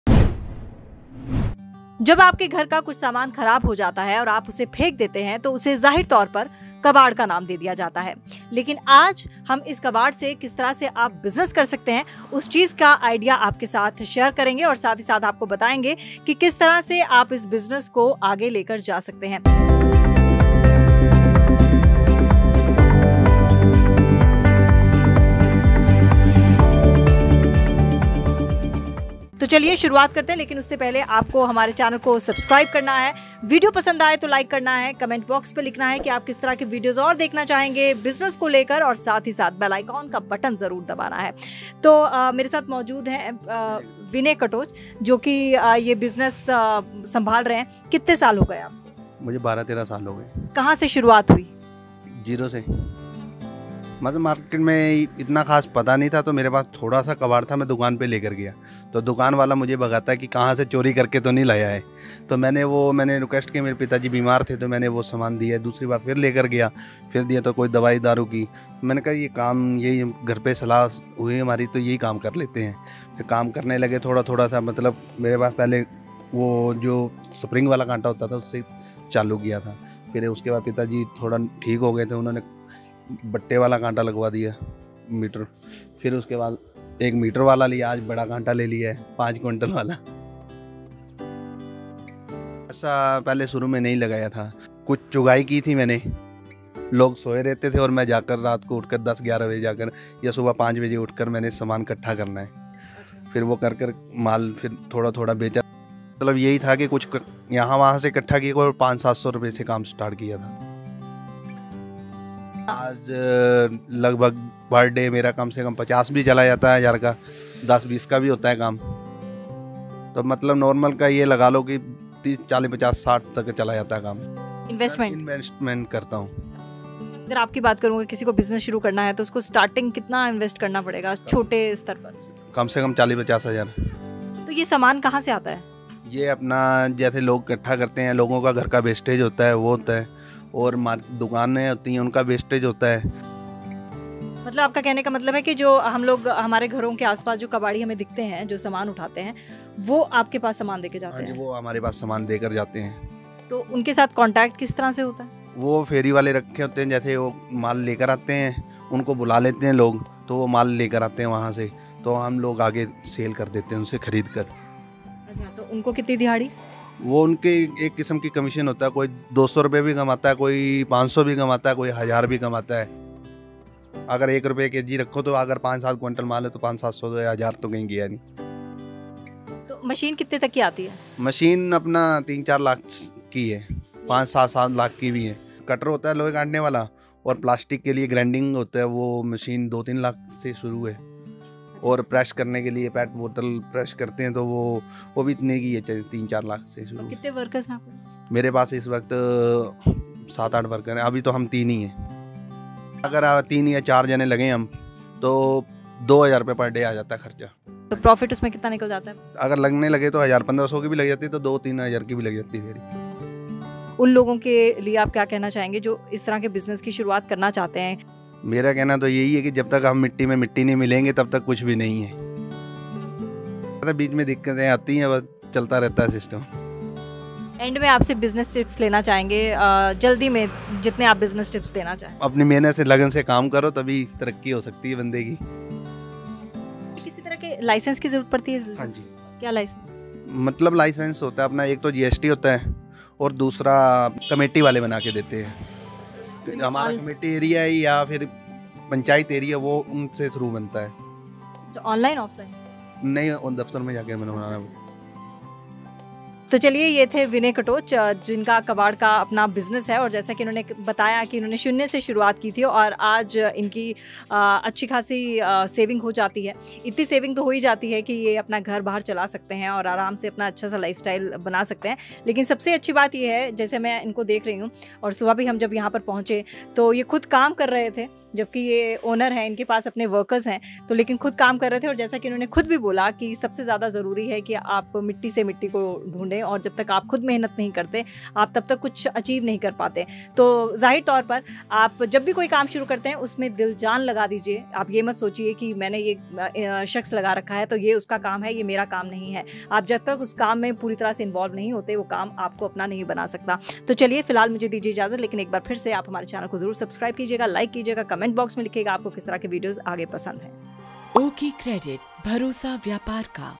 कबाड़ से कमाएं लाखों | शून्य से करें शुरुआत | Business Idea
ये एक सफल बिज़नेस की कहानी है, ये कहानी है उस शख्स की जिसने बिज़नेस की शुरुआत की थी तब उसके पास कुछ नहीं था यानी सफर शून्य से शुरू हुआ और आज एक मुकाम तक पहुँच गया। कबाड़ के इस बिज़नेस से विनय कटोच ने कैसे सफलता पाई और किन बिज़नेस बिज़नेस टिप्स को अपने जीवन में अपनाया, आइये जानते हैं इस वीडियो में उनकी कहानी उन्हीं की जुबानी। ये कहानी और ये बिज़नेस आईडिया उन लोगों के लिए काफी कारगर साबित हो सकता है जिनके पास इन्वेस्टमेंट के नाम पे ज़्यादा रूपए नहीं हैं लेकिन मेहनत और लगन के दम पर कुछ करना चाहते हैं और एक अलग मुकाम तक पहुँवहना चाहते हैं तो दोस्तों वीडियो को अंत तक जरूर देखें।